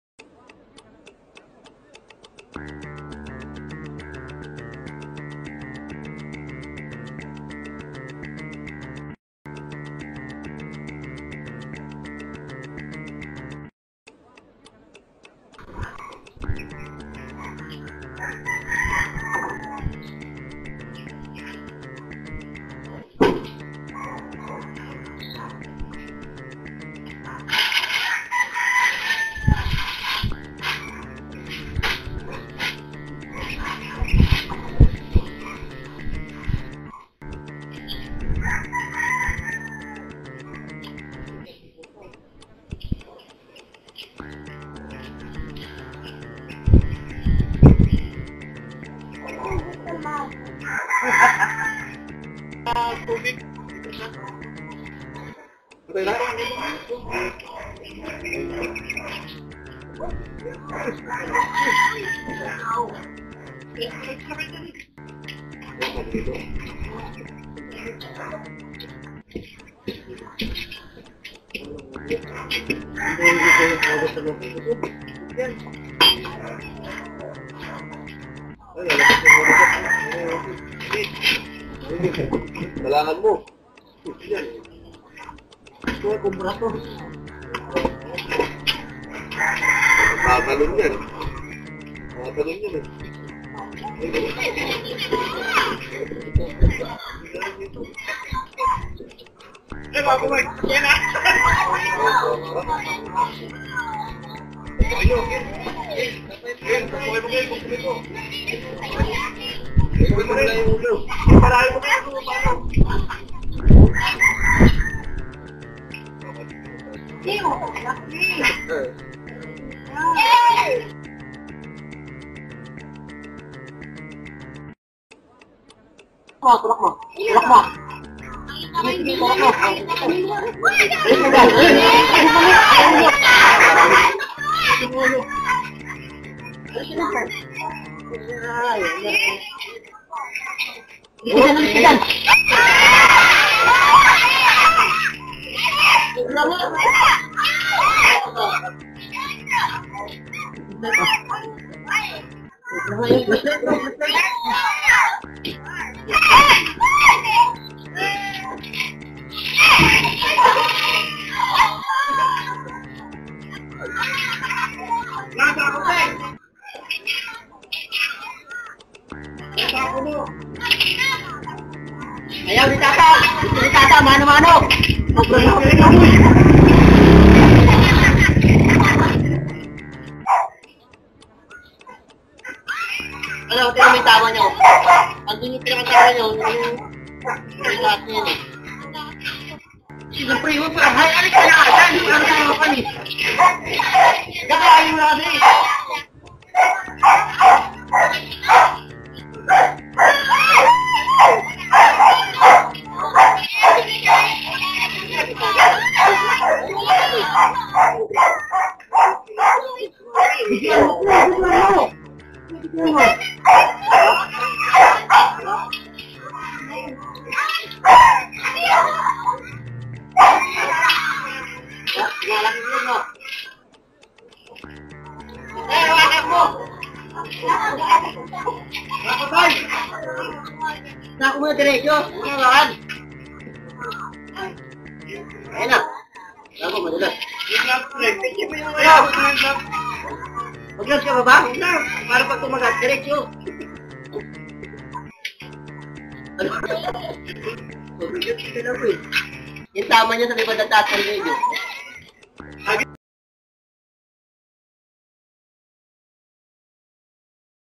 [2024] Dalawang Cobra nahuli sa Villa Annapolis Subdivision Dulong Bayan San Jose del Monte Bulacan
Unang cobra - Feb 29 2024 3:45PM+
Pangalawang cobra - March 2, 2024 4PM+

Location: Phase 2C Orchids St